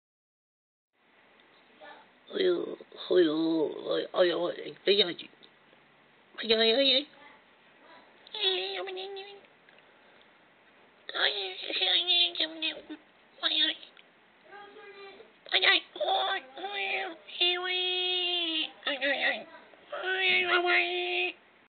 Biggest Zit In The World
Biggest zit ever seen. Don't even ask where these people are from.